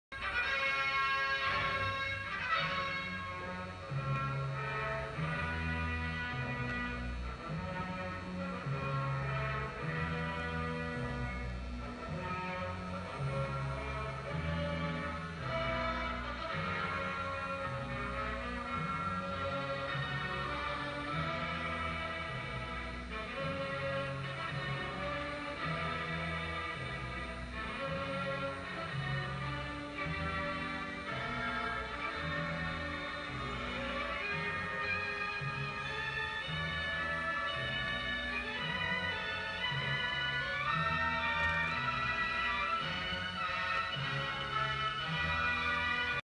Noirestyle
Noirestyle en la pantalla del cine de verano PINAR. Como conmemoración del 10º Aniversario de la página. Un detalle obra de Arqlexdelcine.